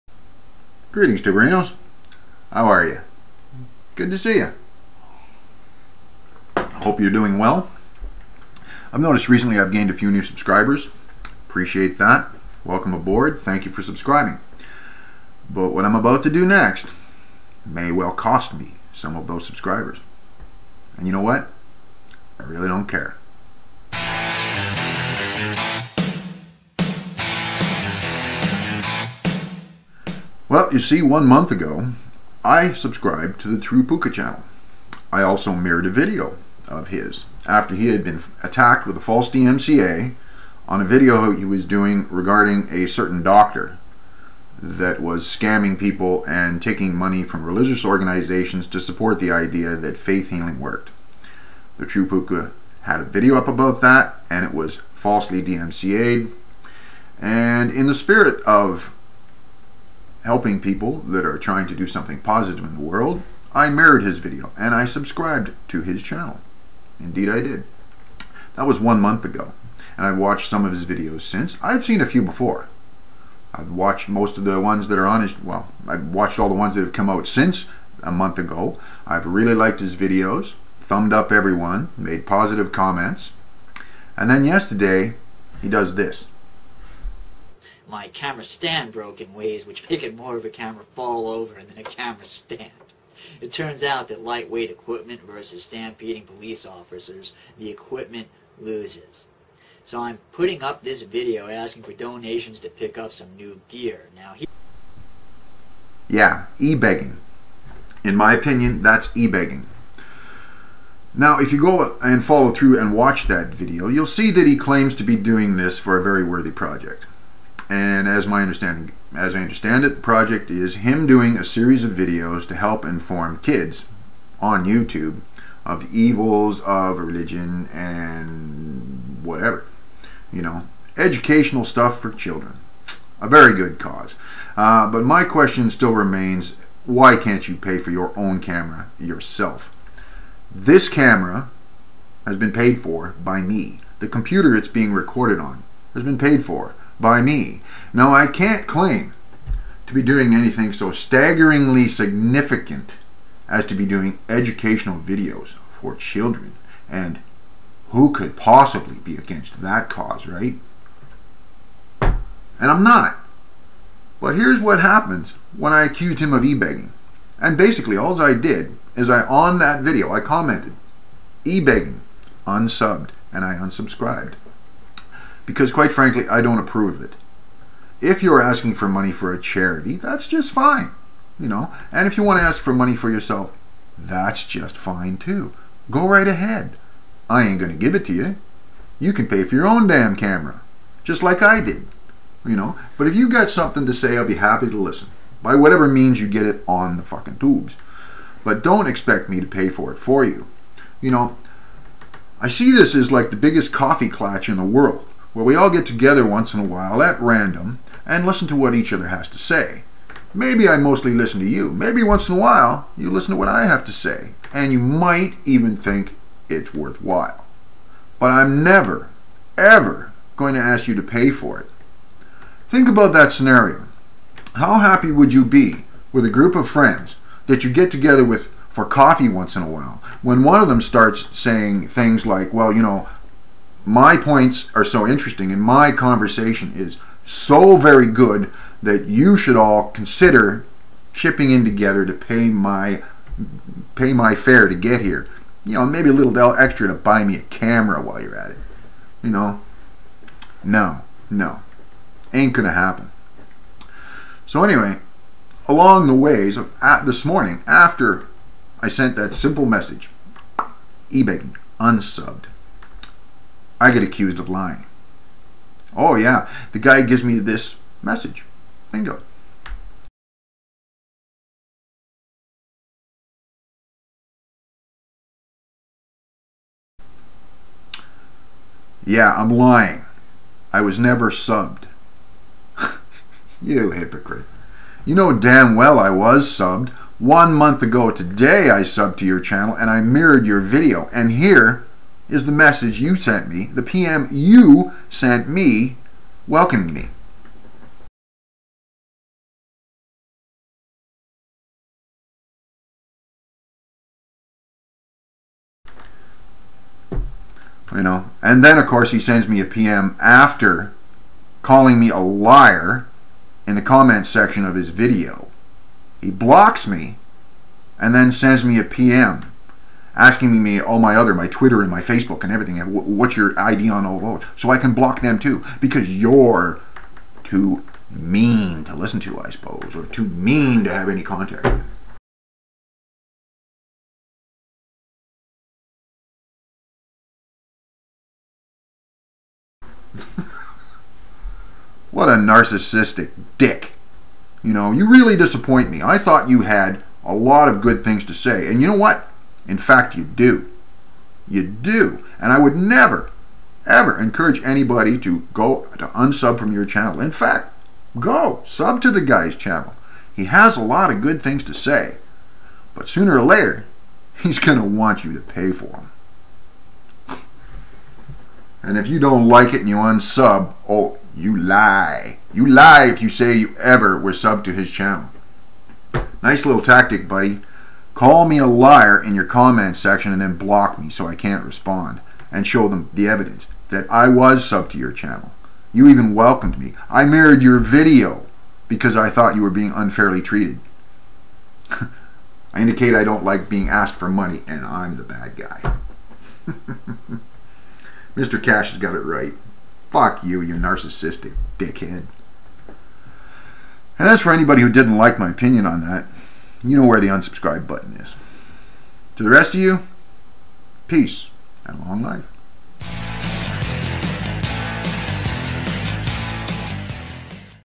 TheTrueEbegger
EDIT: The TruePooka has apologized for his PM to me. I will be responding to this further ASAP. END EDIT.

TheTruePooka has decided to start ebegging and he really doesn't like it to be pointed out.
Apparently he is too high and mighty to be buying his own camera. Perhaps you would like to buy it for him.
The video I am responding to;